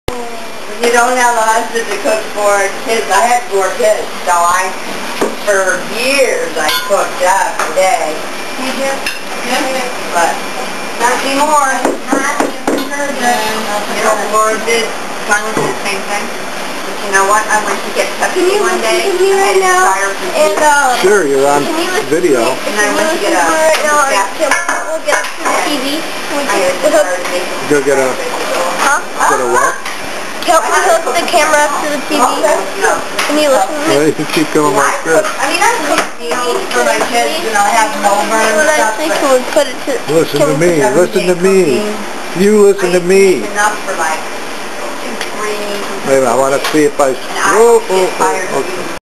Can You Listen to Me??
Noah trying to get some attention in the midst of the usual chaos.